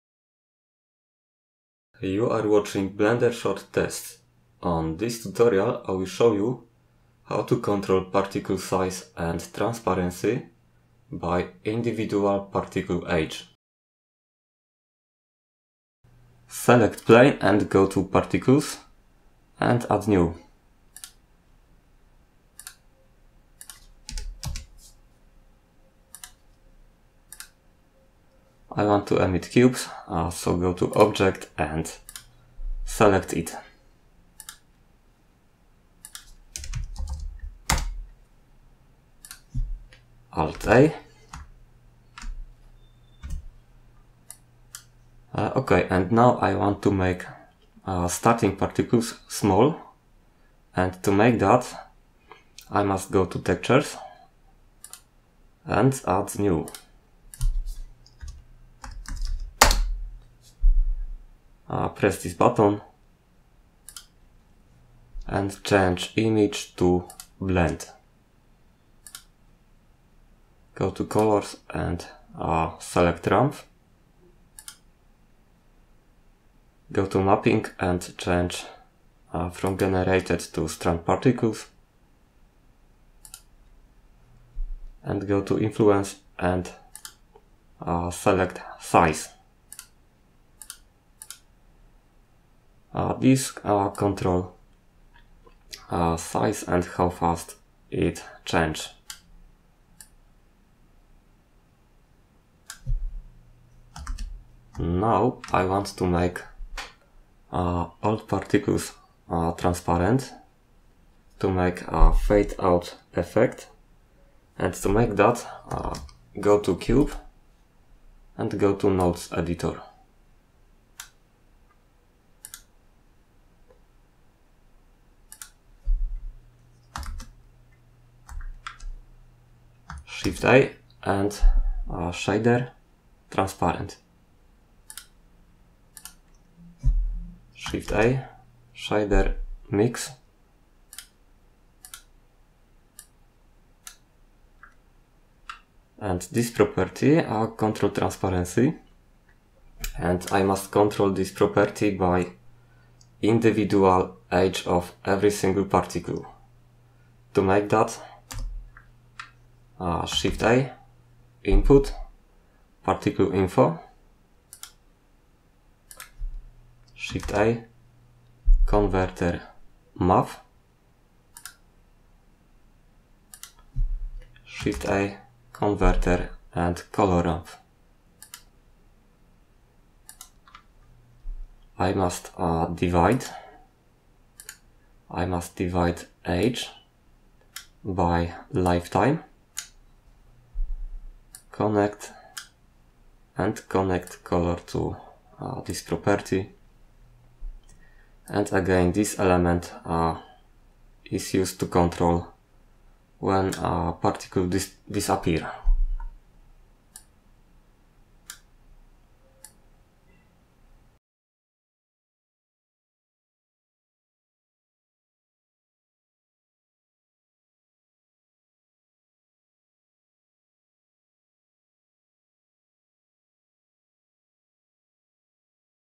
Blender: Particle size and transparency
See how to make appearing and disappearing particles.

00:00 intro
00:20 adding particles
00:30 emiting objects
00:50 size of particles (fade in)
01:45 particle transparency (fade out)
02:30 particle info node